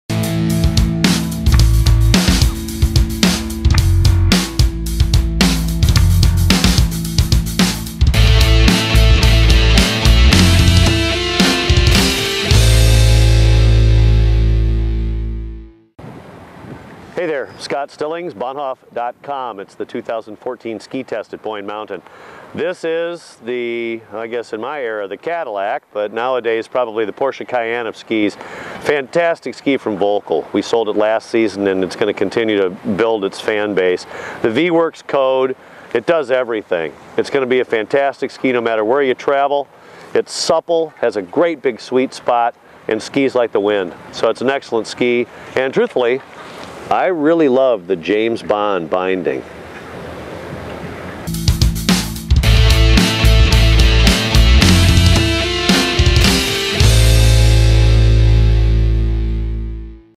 Bahnhof Sport Advisor - 2014 Volkl Vwerks Code - On Snow Ski Test with Scott Stillings
Scott Stillings reports his impressions on the Volkl Vwerks Code skis for 2013-14